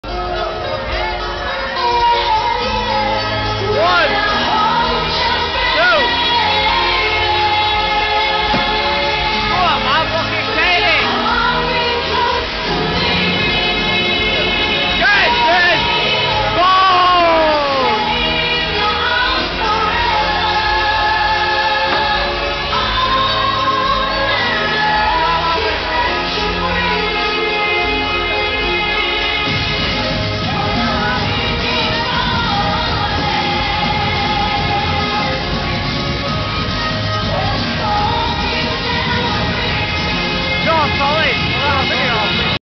Big D Tequila Slammer
Big D absolutly steaming having a tequila slammer with extra salt.